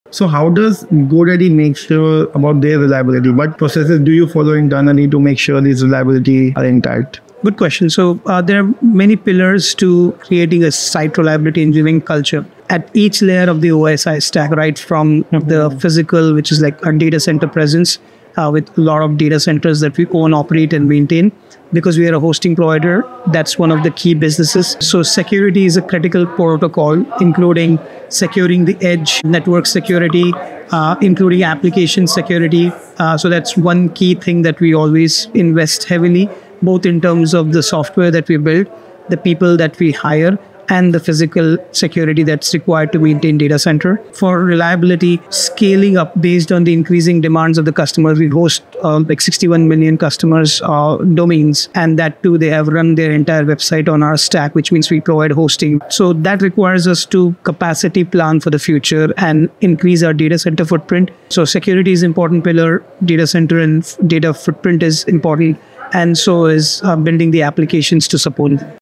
GoDaddy’s secret to hosting millions of websites
GoDaddy builds reliability across every layer — from secure data centers to strong edge networks and smart capacity planning for over 61 million websites.